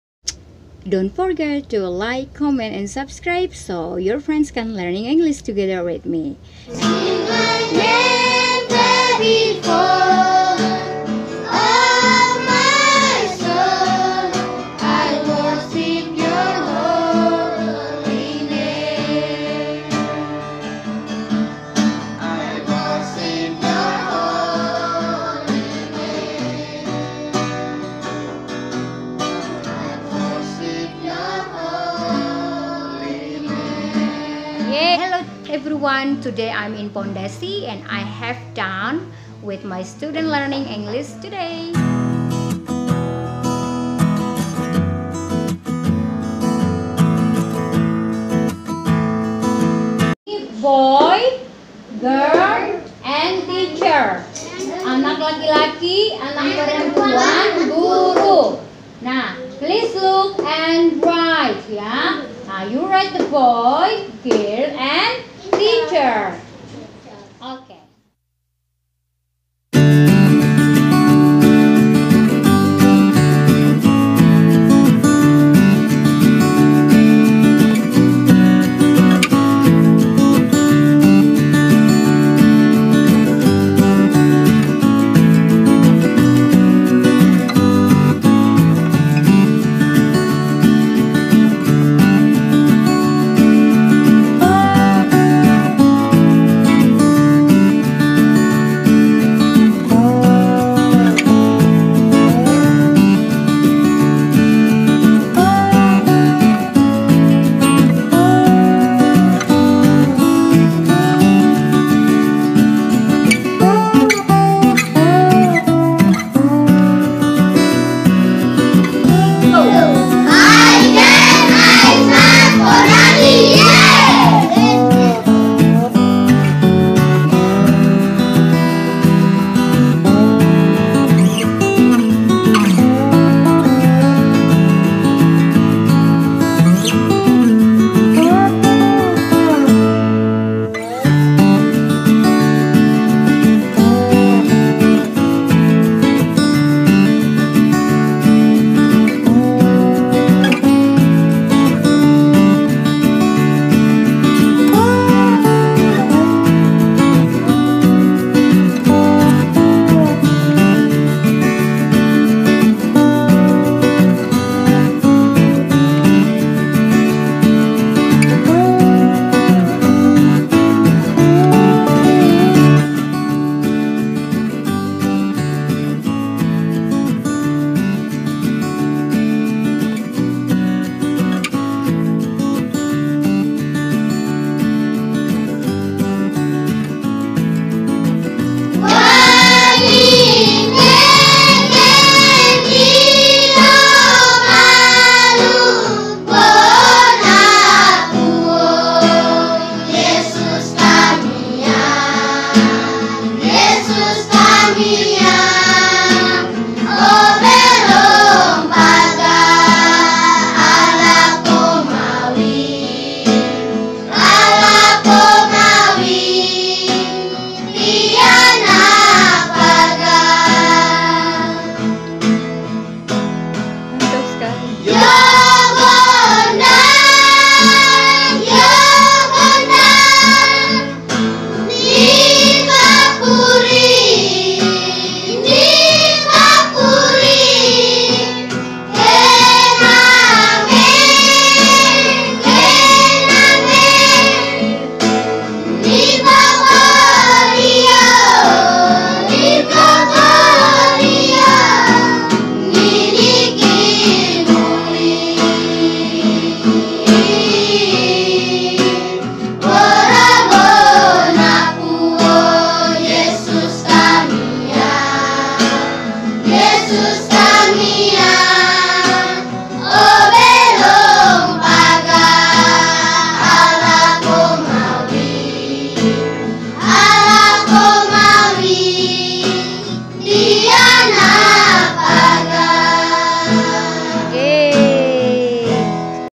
Belajar Bahasa Inggris dan Bernyanyi
Anak anak muridku belajar bahasa inggris, dan mereka pandai bernyanyi juga. Terimakasih anak anak kalian tetap semangat.